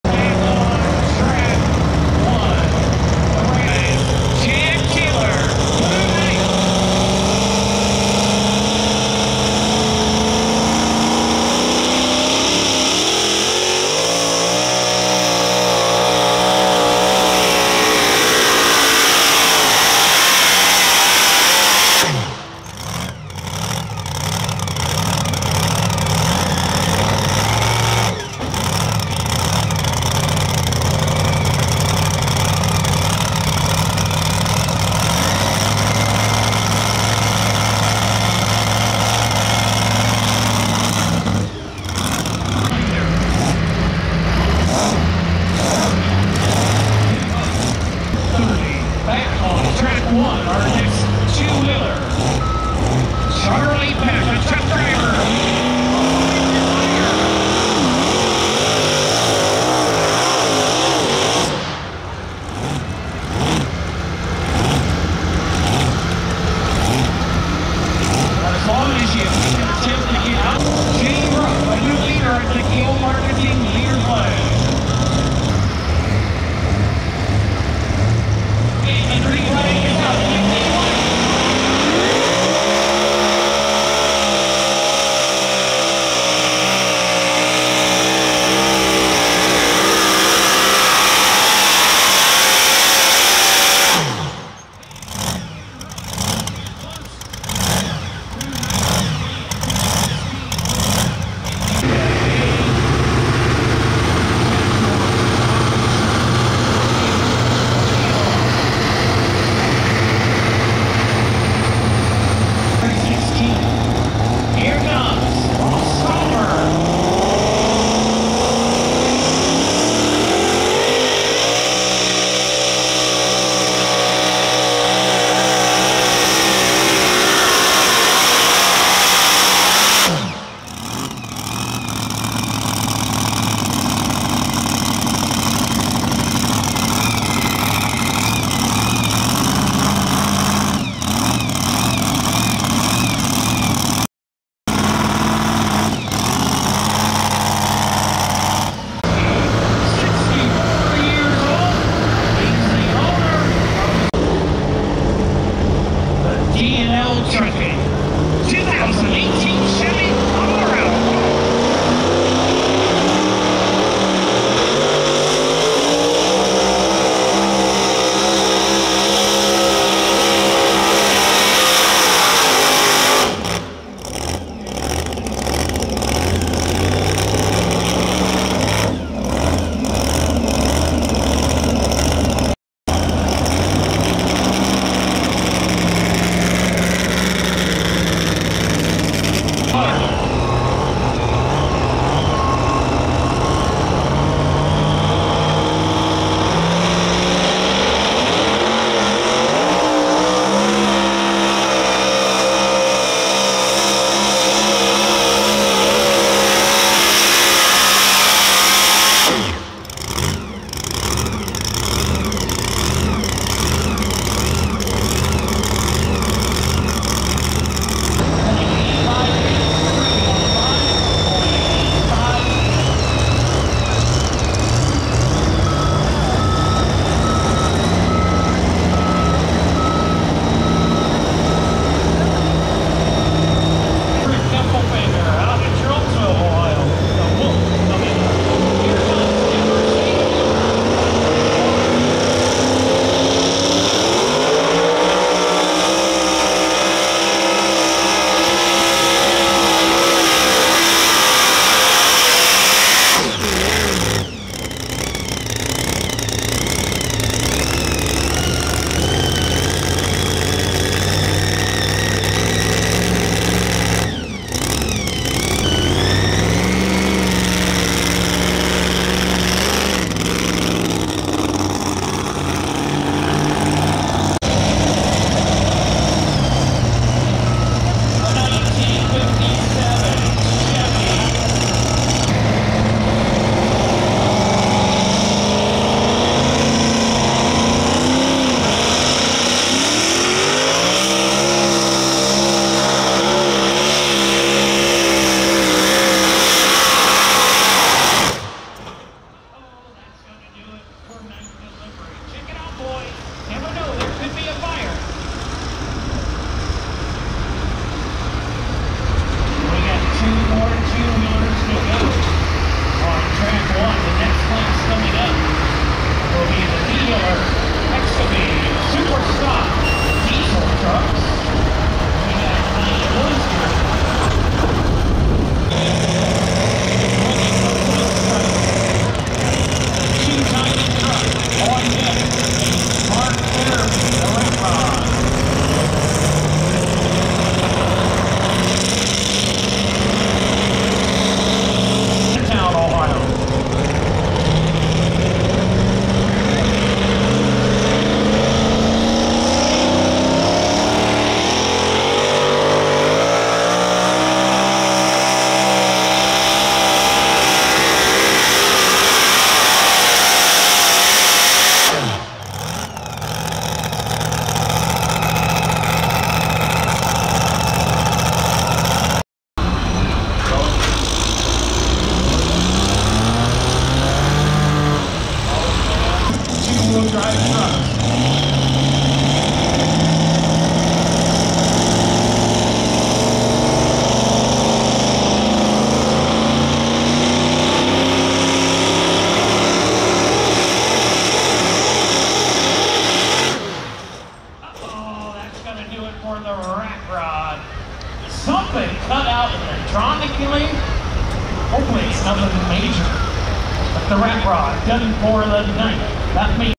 Earth shaking Ear Splitting Super Modified 2wd Pulling Trucks OSTPA Dragway 42 spring Showdown 2024
Truck pulls
Truck pulling
Truck pull videos
Tractor pulls